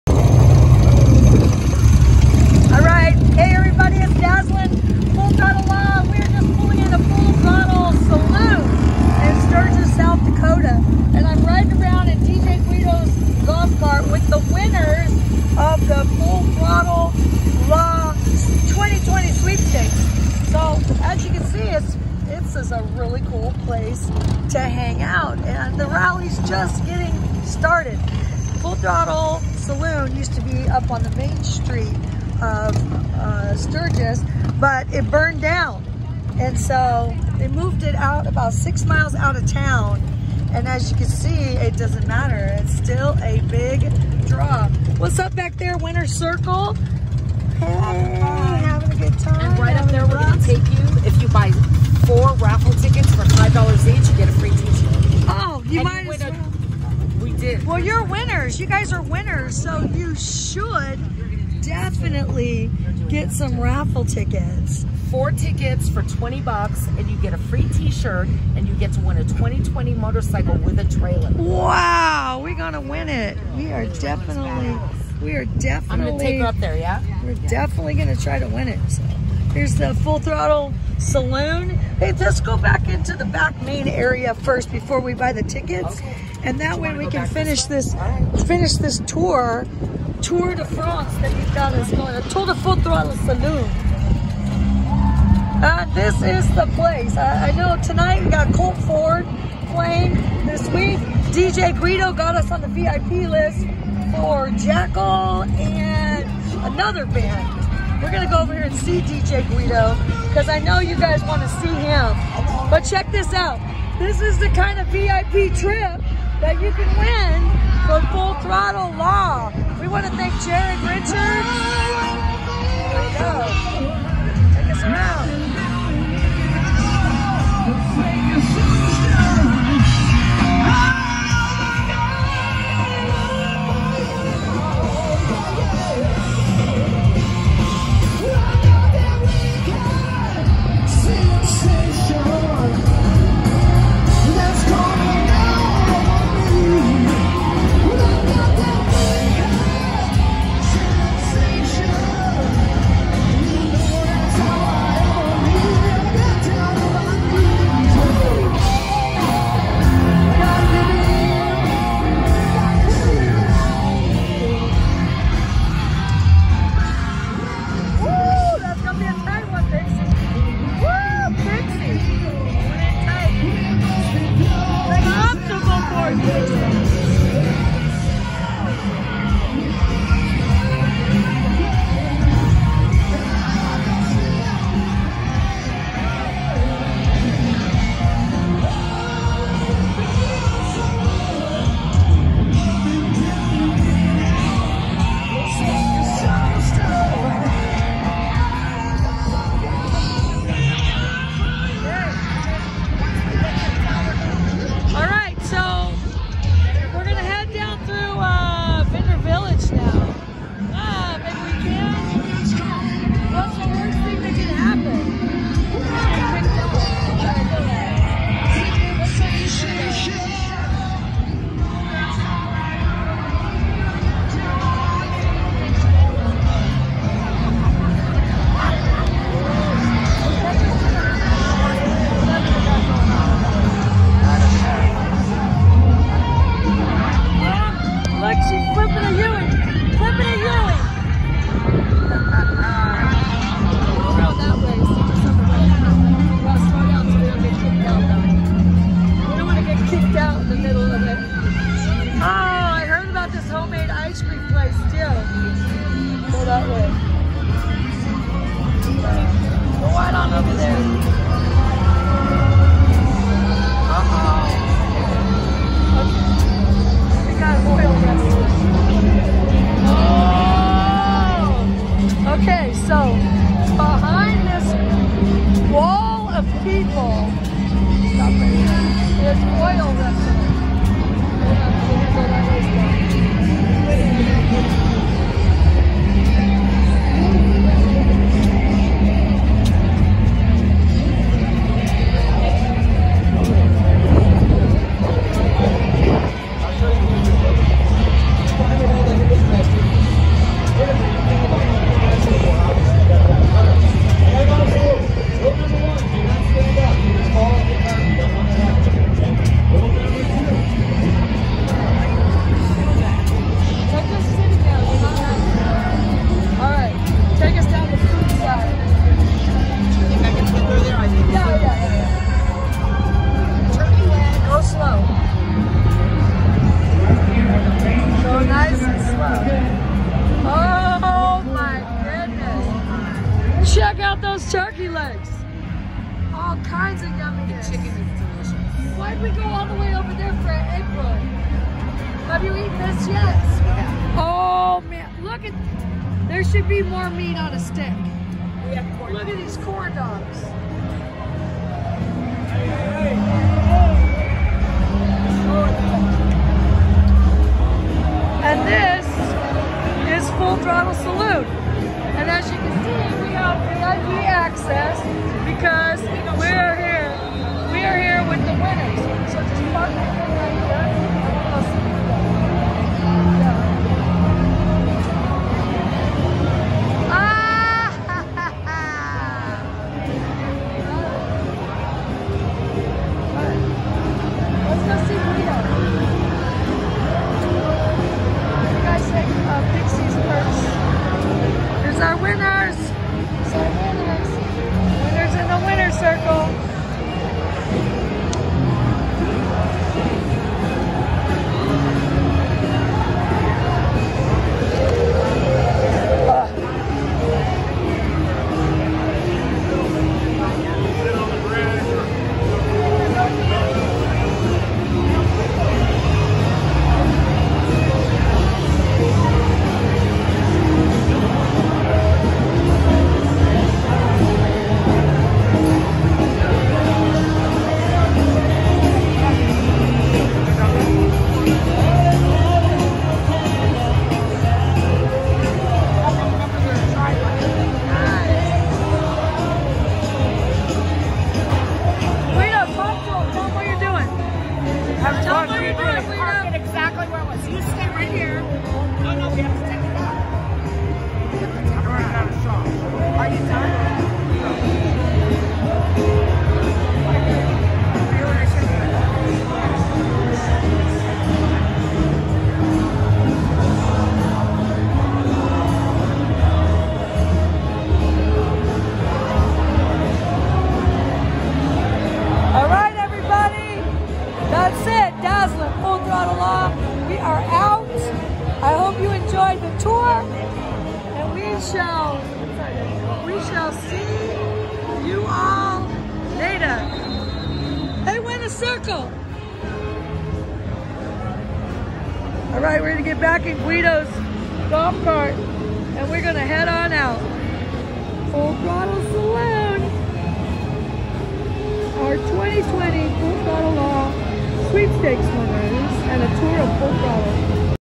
Behind the Scenes at Sturgis 2020 and the Full Throttle Saloon
Full Throttle Law 2020 Sturgis Trip Winners take us on a tour of FTS!